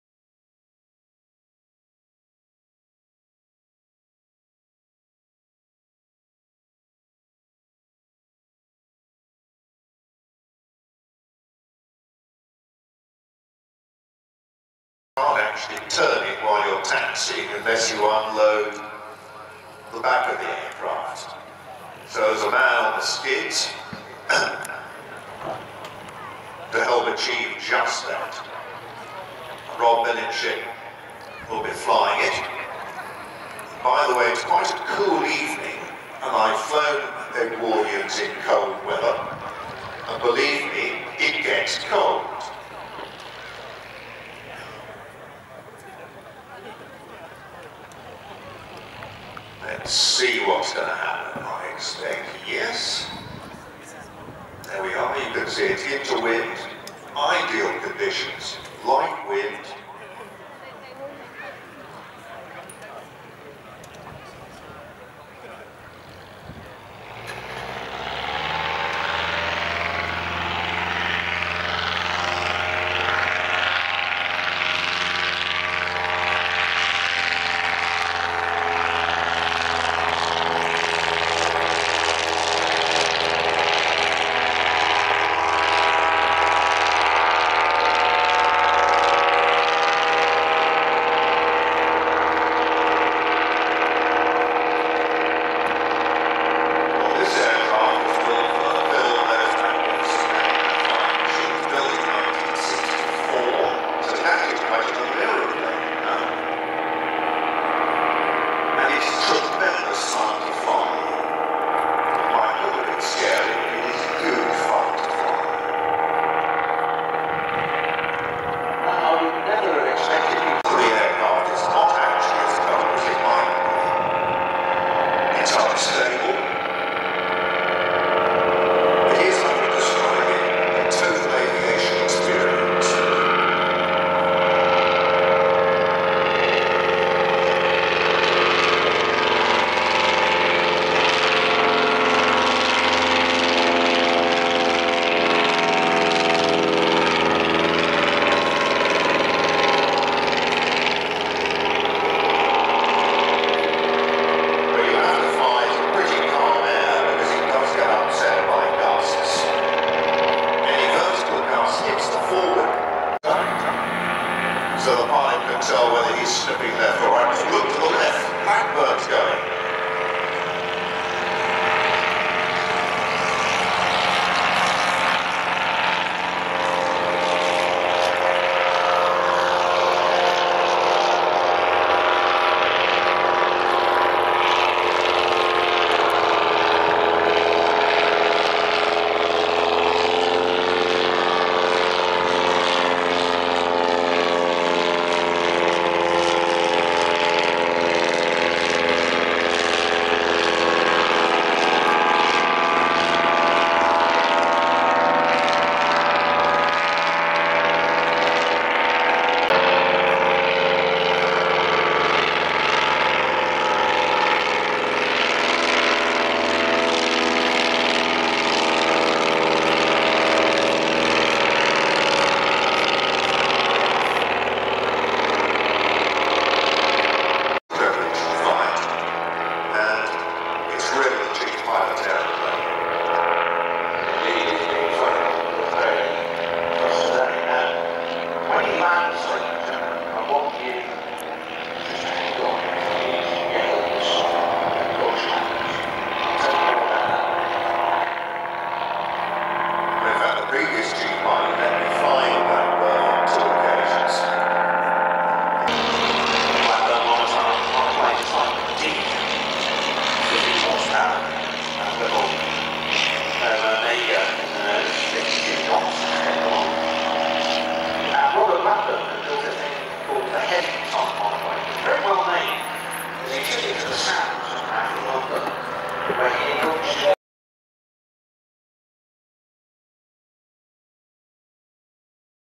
SHUTTLEWORTH RACE DAY OLD WARDEN BEDS 07.10.2018 THE EDWARDIANS.
The Bristol BOXKITE 1910 (REPLICA) built for the film `Those magnificent men in and there flying machines 1965 film the last Boxkite flying.
1910 Deperdussin.